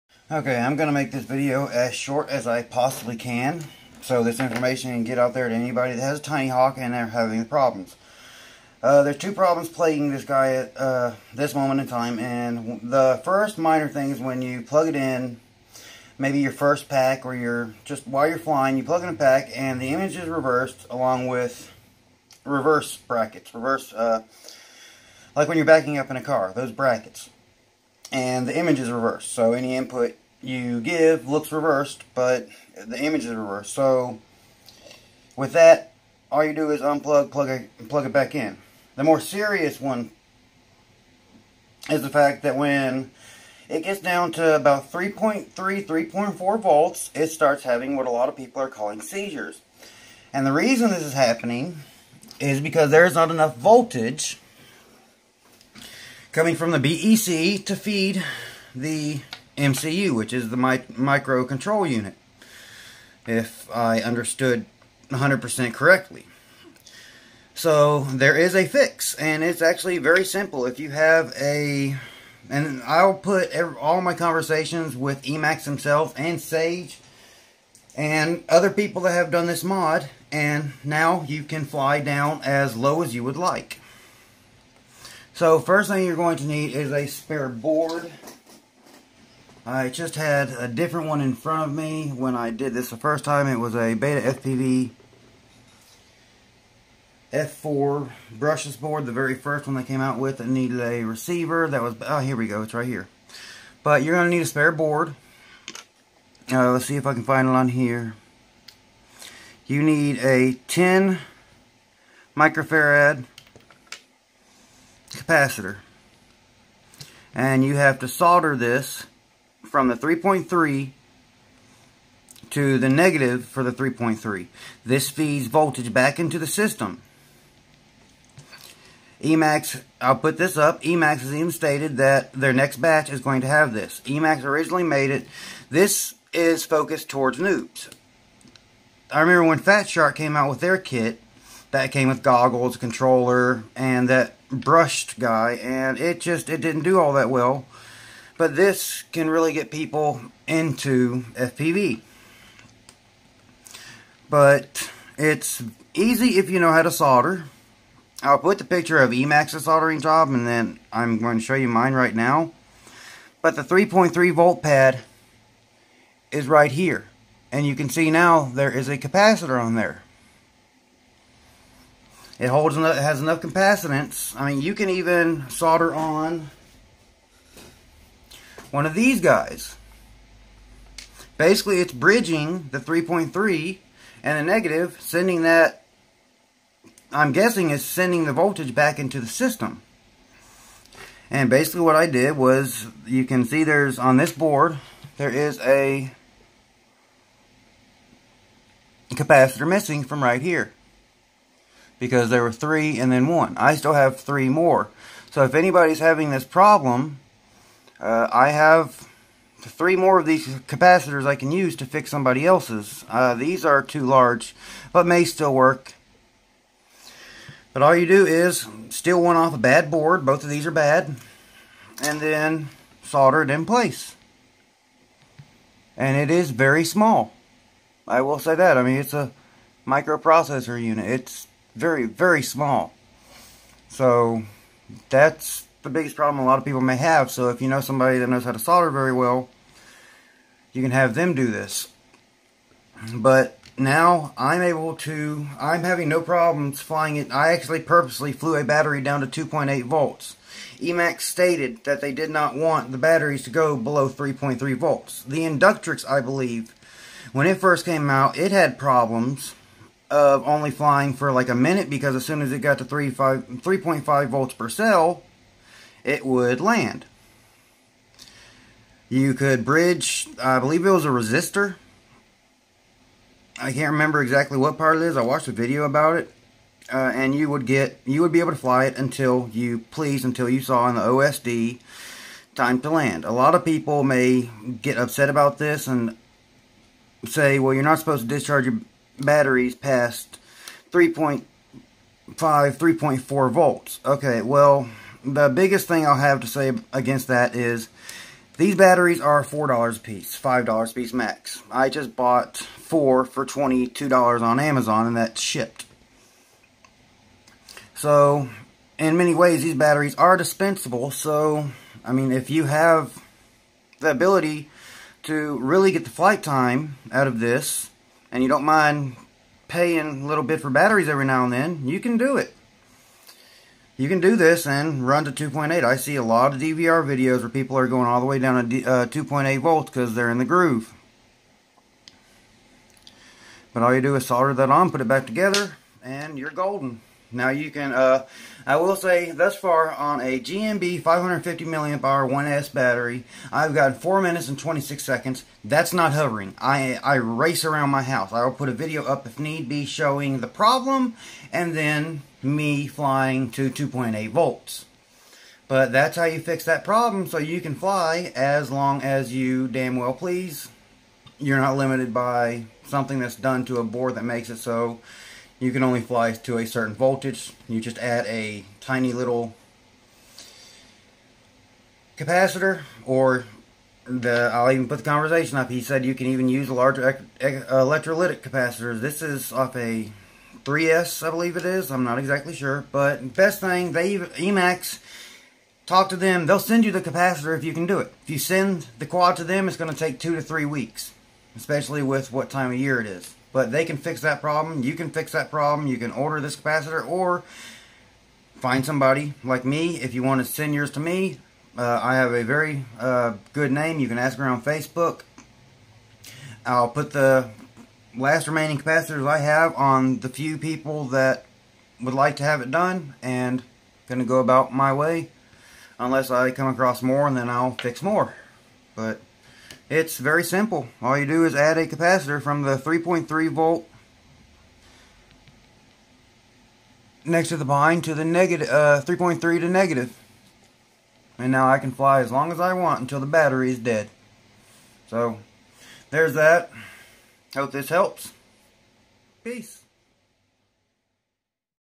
Emax Tiny Hawk Voltage Issue FIXED/how to
Is your Tiny Hawk acting very weird at 3.3 to 3.4 volts?  wanna fly for 4 minutes.  a simple but somewhat difficult mod can be done to give you all the flight you're willing to push your pack.